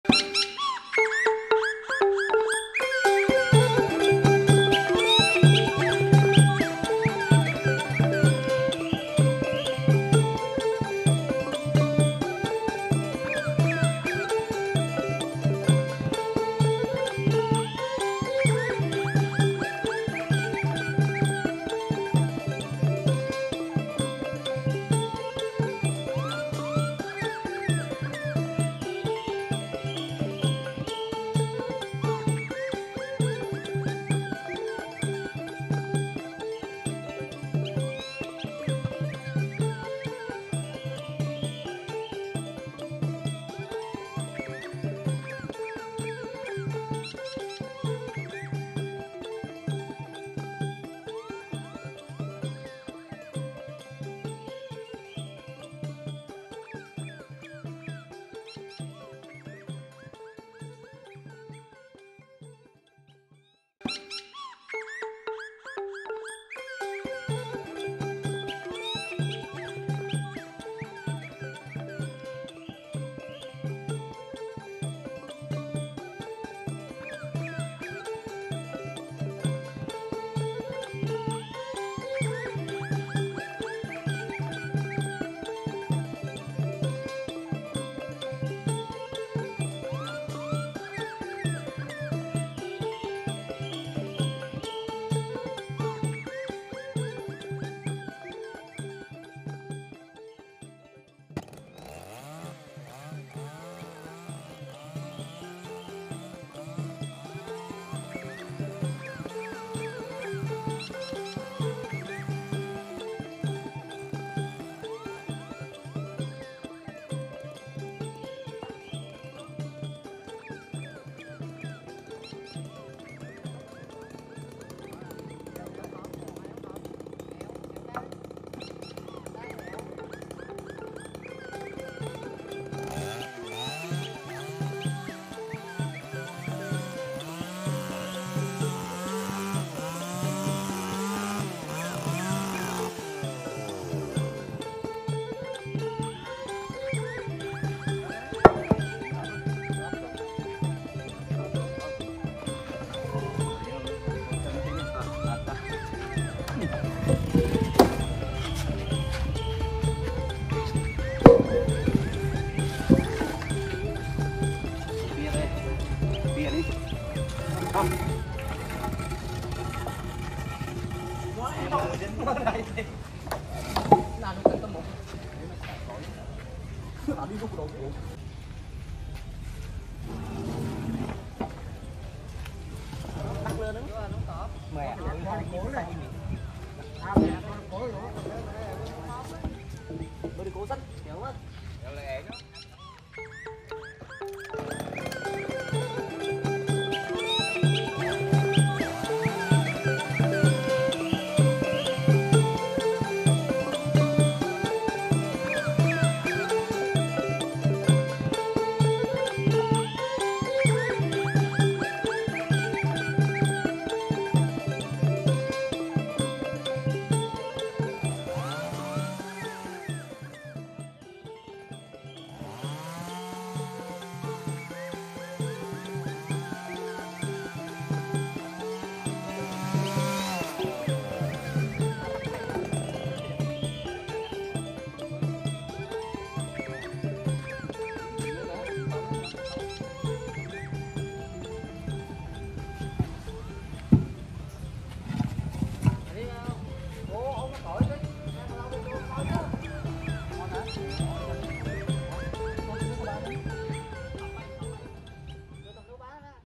Chùa Khleang Sóc Trăng đóng GHE NGO mới với gỗ cây sao quý|STV Khám phá
Kênh chính thức của Đài Phát Thanh & Truyền Hình Sóc Trăng.